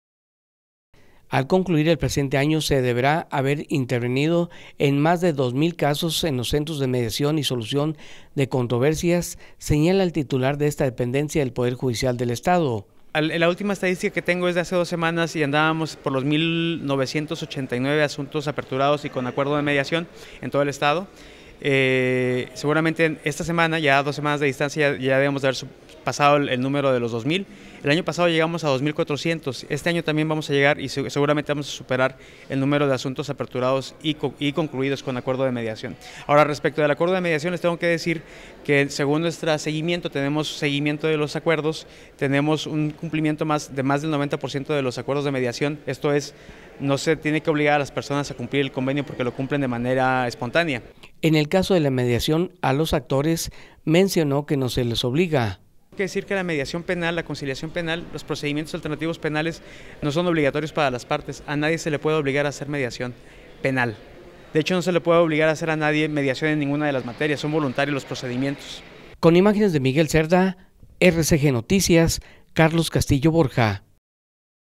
Serán más de 2 mil los casos presentados en centros de mediación
Al concluir el presente año, se deberá haber intervenido en más de 2 mil casos en los centros de mediación y solución de controversias, señaló el titular de esta dependencia, del Poder Judicial del Estado.